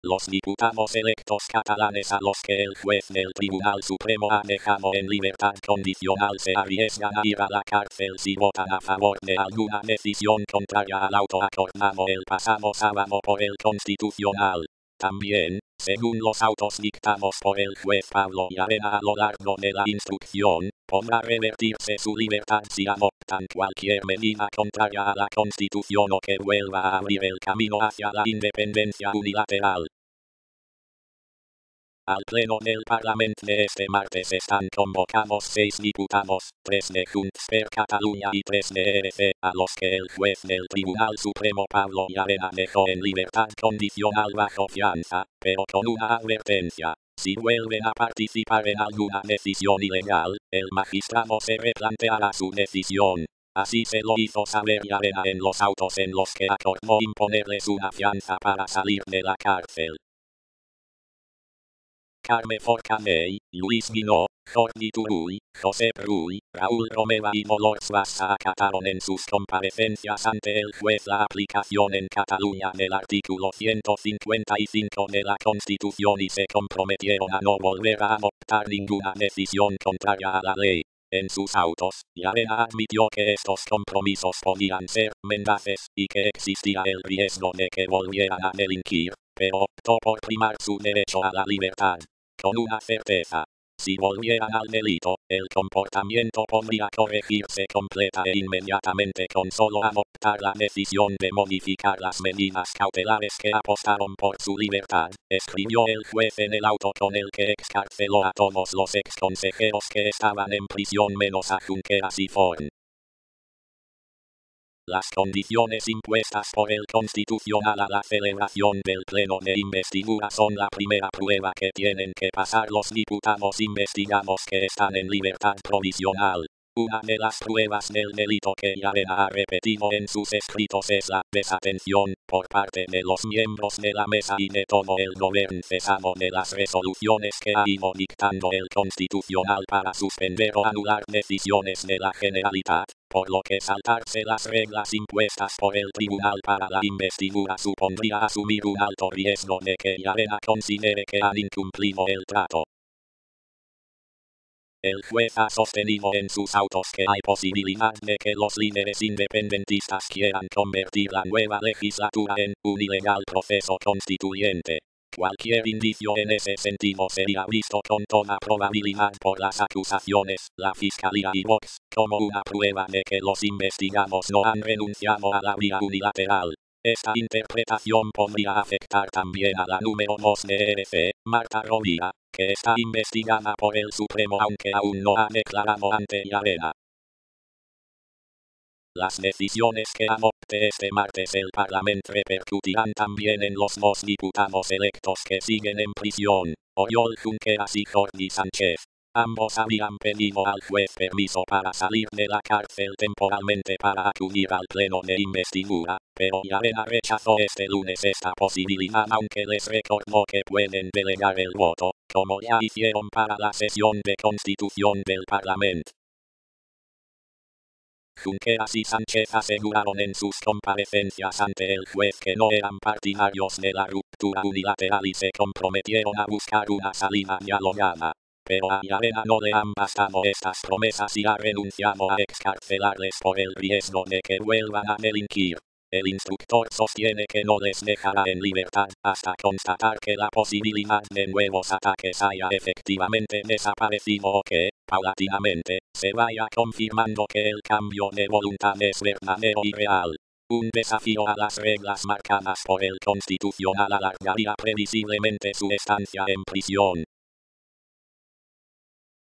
Los investigados se arriesgan a ir a la cárcel si ignoran al Constitucional
Los diputados electos catalanes a los que el juez del Tribunal Supremo ha dejado en libertad condicional se arriesgan a ir a la cárcel si votan a favor de alguna decisión contraria al auto acordado el pasado sábado por el Constitucional. También, según los autos dictados por el juez Pablo Llarena a lo largo de la instrucción, podrá revertirse su libertad si adoptan cualquier medida contraria a la Constitución o que vuelva a abrir el camino hacia la independencia unilateral. V: ves_001799.mp4